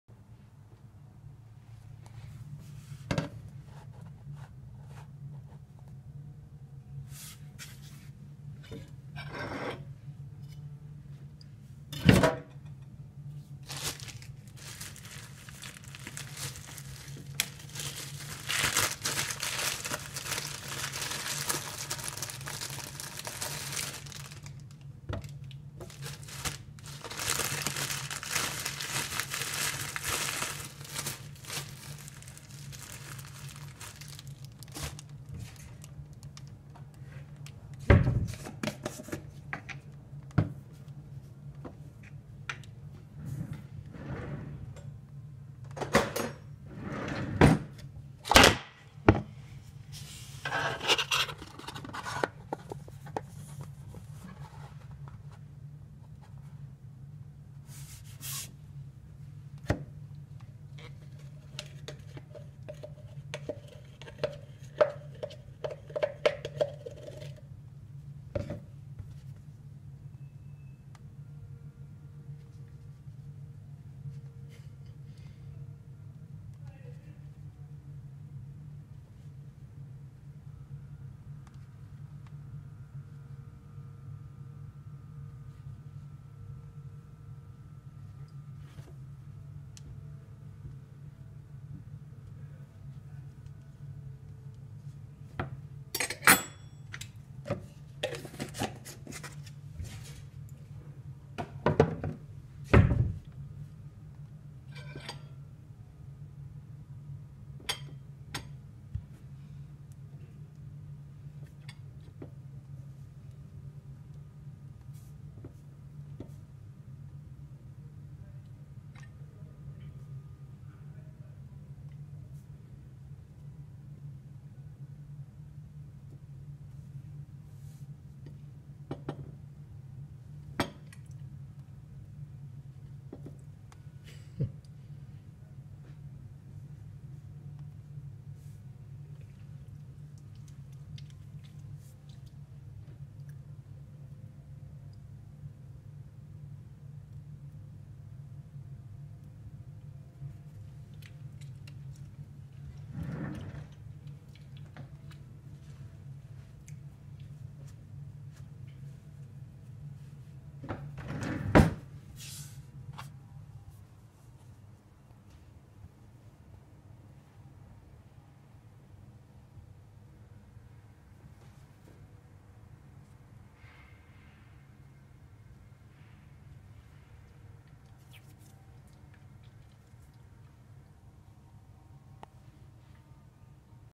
vlog #83 peanut butter and banana sandwich
preparing a peanut butter and sliced banana sandwich.  peel the banana, and then cut it into slices.  then spread peanut butter onto a slice of bread.  then place the sliced banana pieces onto the peanut butter.  then place a second slice of bread onto that banana slices.  Voila!